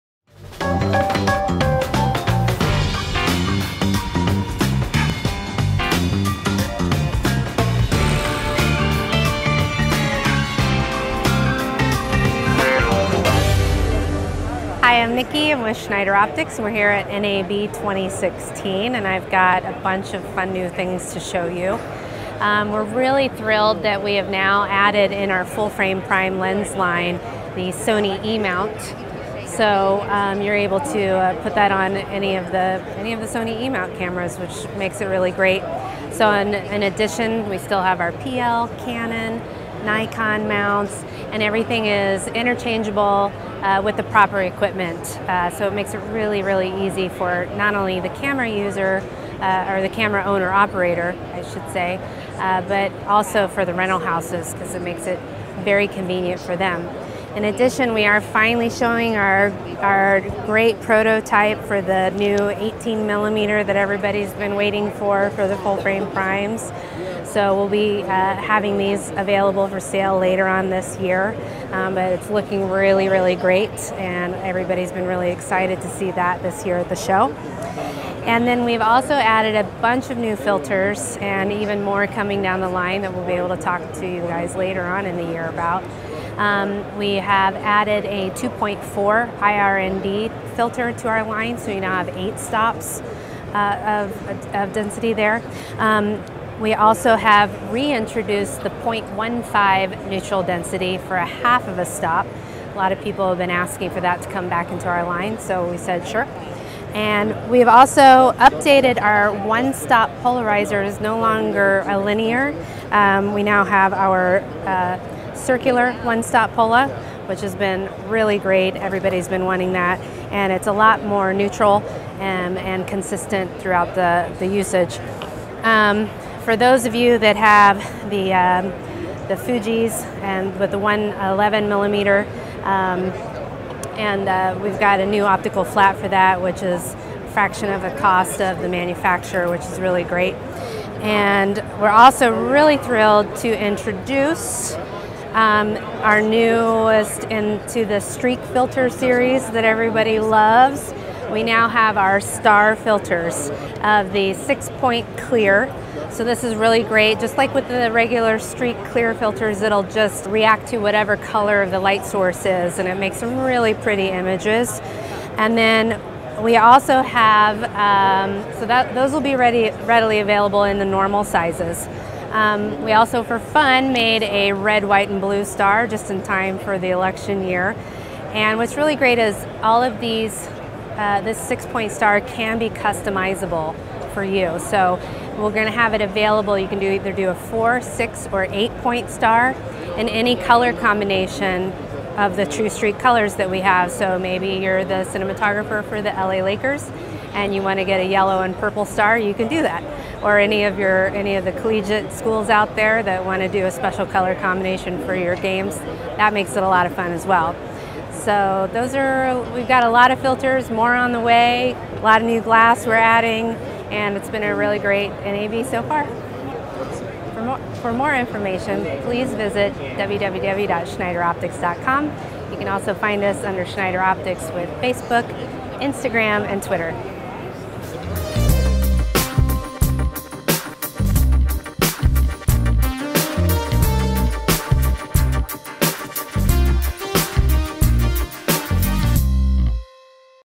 Schneider Optics FF-Primes & Filters at NAB 2016
An interview from the 2016 National Association of Broadcasters Convention in Las Vegas with Niki Mustain of Schneider Optics. Schneider Optics has been producing the highest quality optics on the market for over 90 years, offering solutions for large-format photography, photo enlarging, motion picture projection, optical filtration, and industrial applications. In this interview  Niki talks with us about Schneider Optics Xenon FF-Primes (25,35,50,75 & 100), now available in Sony E-mount as well as Canon EF, Nikon F, & PL. Renowned MPTV lens filters include True-Pol®, Classic Soft®, True-Streak® new Confetti, and 1-Stop Circular Polarizer.

This will give users high performance lenses  with which to capture images from the full-frame sensor that the a7 series provides without busting the budget. Schneider currently offers primes in 25mm, 35mm, 50mm, 75mm, and 100mm focal lengths with 18mm and 135mm coming in 2016. They are specifically designed in Germany for digital cinematography applicaitnos, and Schneider is focusing (pardon the pun) on the future by gearing these lenses for resolution at the 4K and above range.

Schneider’s new Platinum Series IRND filters limit the light striking the camera’s CCD or CMOS imager to the visible spectrum. By carefully calculating the cutoff frequency in nanometers, we have been able to produce a near-infrared cut filter that lets users of high definition cameras get the most out of their camera gear. This means the benefit of an extended color gamut without the worry of unwanted false colors. Eliminating the near-infrared light leakage lets the camera maintain true color rendition in the blacks while maintaining high MTF of its lenses and camera system.

Schneider Platinum Series IRND filters are free of off-axis color shift regardless of the focal length and can be stacked without introducing reflections common in dichroic filters. They can also be used as a standard ND filter with all HD video and film cameras. These filters are available in all standard video and cine sizes, including: 4x4, 4x5.65, 5x5, 5.65x5.65, 6.6x6.6 sizes plus rounds in 138mm, 4.5” and Series 9. This filter is available in 0.3, 0.6, 0.9, 1.2, 1.5, 1.8, 2.1 and 2.4 stop densities. Higher densities can be achieved by stacking two or more filters.

The 111mm Clear filter is made for the Fuji Cabrio Lens Series that have the M111 front diameter thread. Schneider Optical Flats protect expensive front elements and other filters while providing the truest image, free of unwanted color shifts. Made from Schneider’s clear optical glass, these filters minimize or eliminate the need for laboratory color correction caused by filter variations. Both the front and rear surfaces of these solid-glass (non-laminated) filters receive Schneider’s hard anti-reflective (AR) coating, which measurably improves light transmission.

The original True-Streak filters re-create the classic Anamorphic streak effect without the use of expensive anamorphic lenses. Traditionally the Blue True-Streak filters are used to create this effect, and to not limit your imagination we also have 6 other colors to choose from, as well as Clear. As an expansion of the True-Streak family, we have also created the Confetti True-Streak and the Star True-Streak to really bring your vision into a full spectrum of colors.